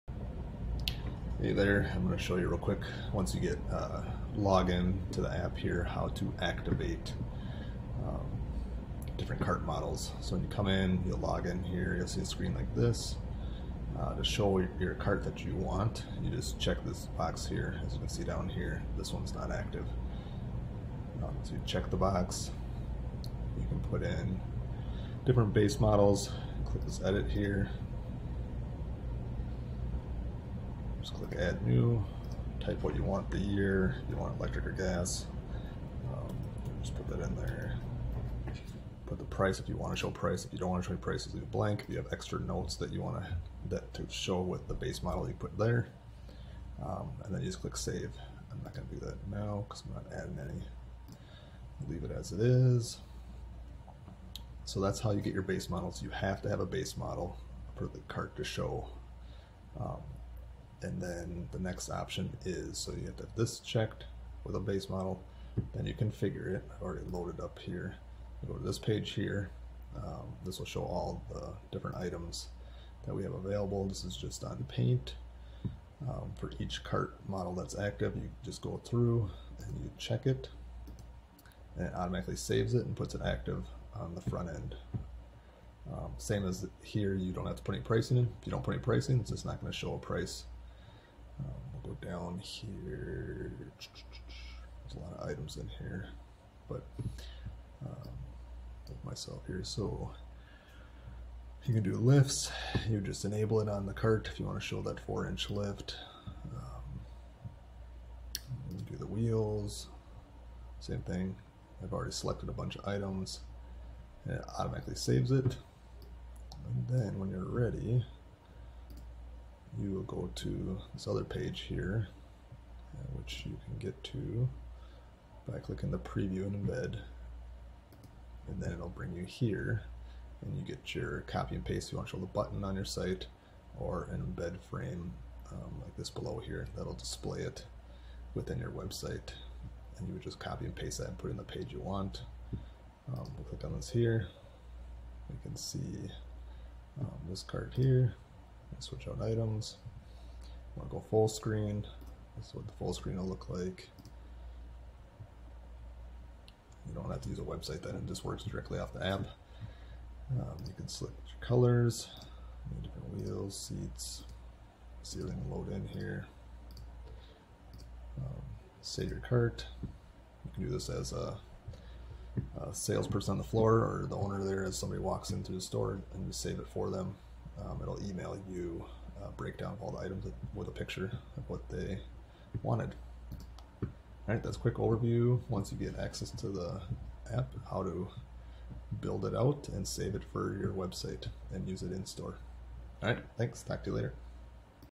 Golf Cart Builder App - Walk Through in App
A walk through for golf cart dealers on how to edit their golf cart builder and update anytime.

7 EASY STEPS:
1. Log into the Build Your Golf Cart software desktop app.
2. Select your Cart Models (check box).
3. Enter Base Models (year). Must have one item in the Base Models to show the builder on the front end.
4. Select Configure button and check items you want to show on your golf cart builder. It auto saves as check the items.
5. Go back to the Home tab.
6. Select Preview & Embed.
7. Copy your link and/or embed code and place on your website!